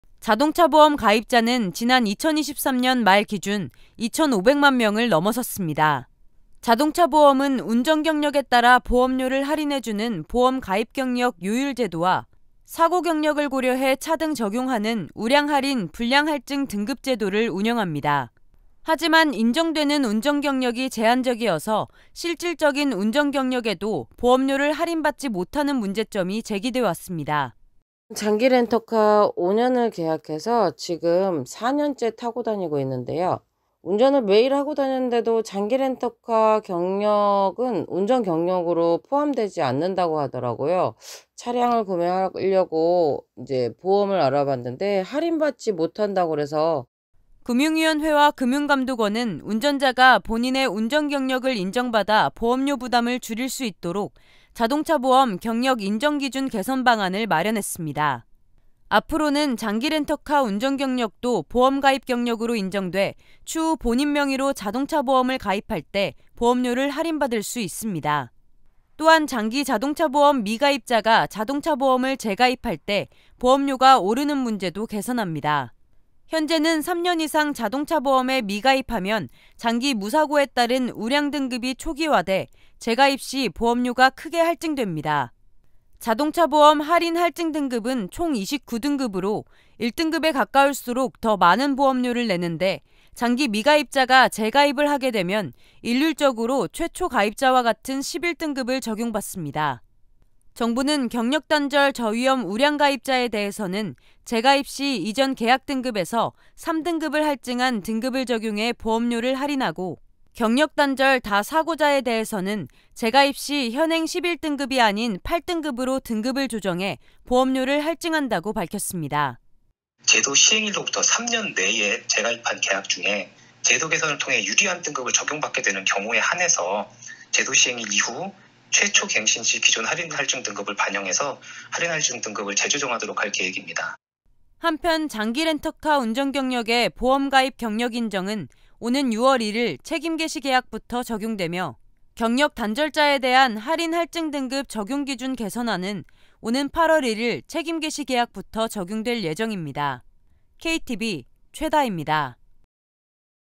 장기 렌터카 운전 경력, '보험 경력'으로 인정
오랫동안 사고없이
운전을 해왔다면
자동차보험료에
반영되는 것이
합당할 텐데요.

그 동안 장기
렌터카 이용자나
차보험 경력이
잠시 단절된
무사고 운전자는
불이익을
당해왔습니다.

정부가 이들의
경력을 인정해주는
개선책을 내놨습니다.